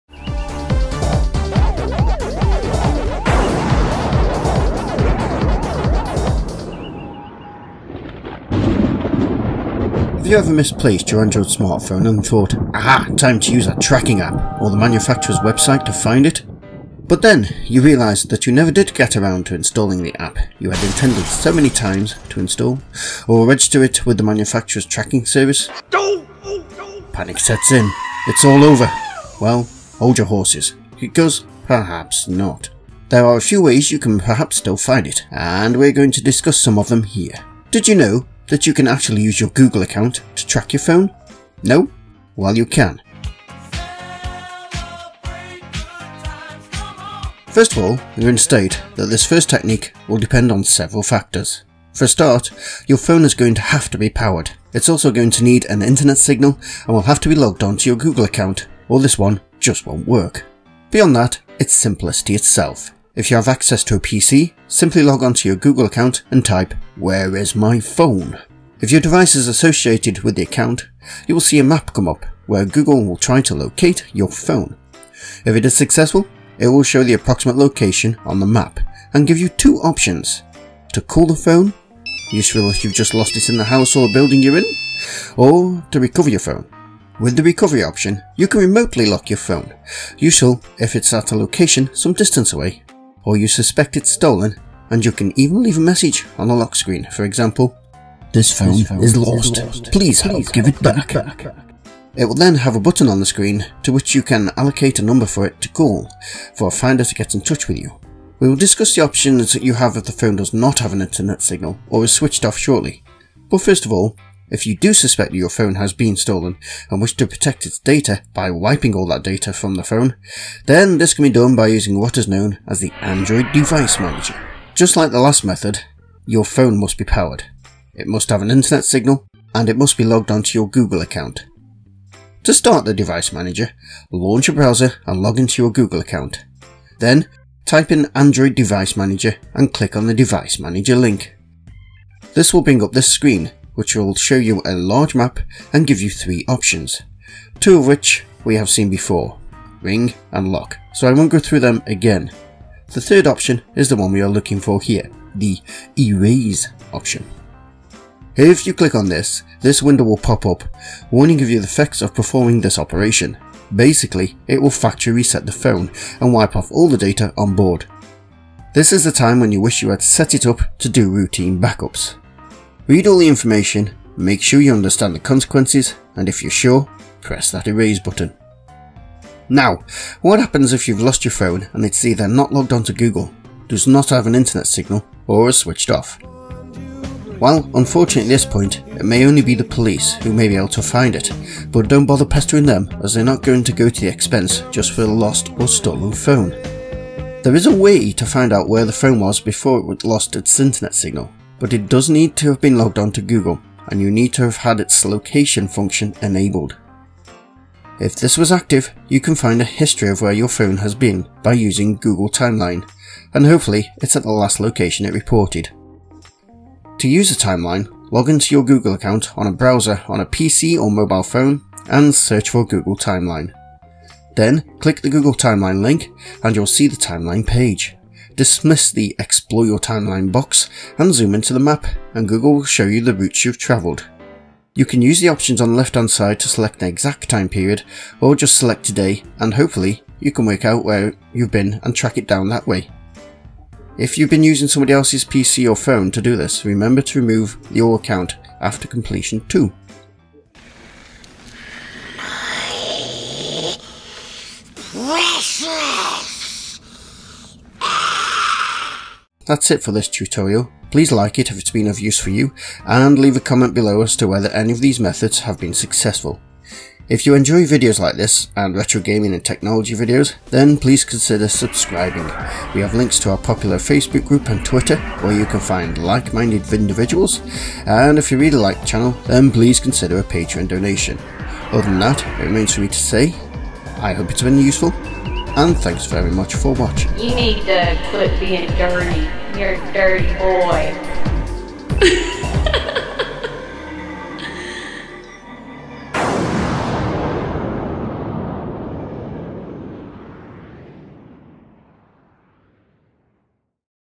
How To Find Your Android Phone / Tablet Without A Tracker App ( Tutorial )
In this tutorial, we are looking at ways you can find your Android phone or tablet, if you don't have a tracker app involved.  I also show a technique for tracking your phone if it is switched off, doesn't have an internet signal.